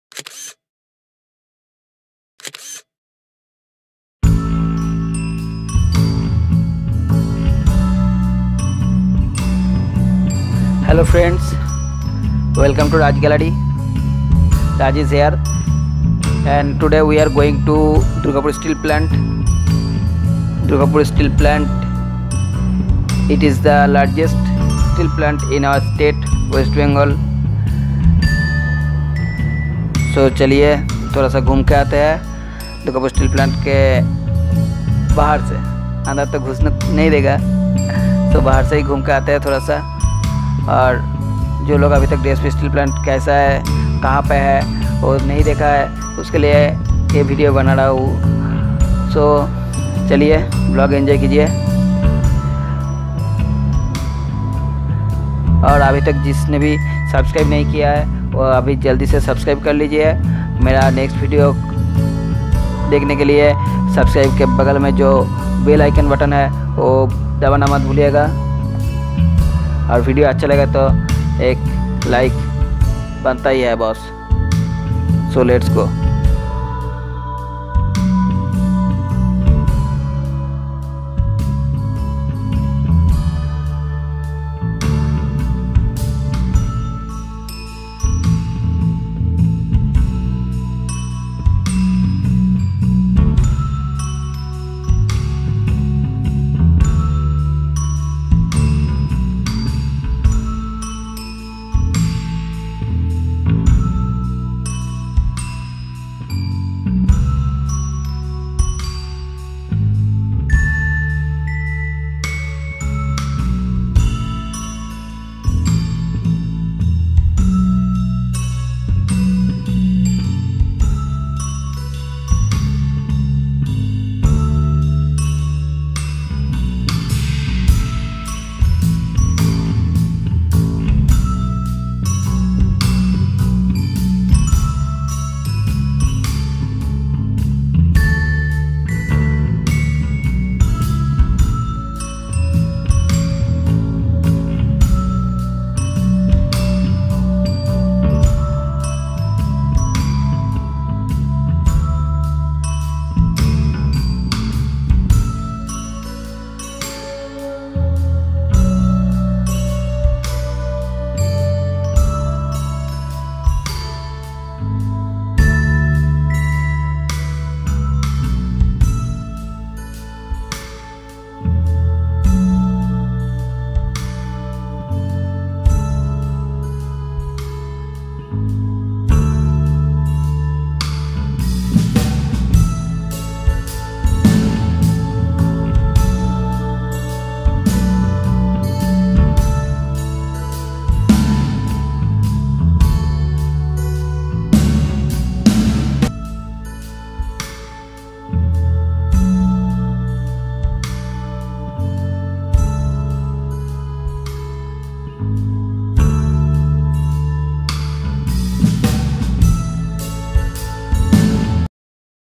DSP // DURGAPUR STEEL PLANT// VLOG 3
Durgapur Steel Plant is one of the integrated steel plants of Steel Authority of India Limited, located in Durgapur, in the eastern Indian state of West Bengal.
DSP is situated about 158 km from Kolkata.
It is situated on the banks of the Damodar River in the Bardhaman District in the growing industrial city of Durgapur. The Grand Trunk Road and the main Kolkata-Delhi railway line pass through Durgapur.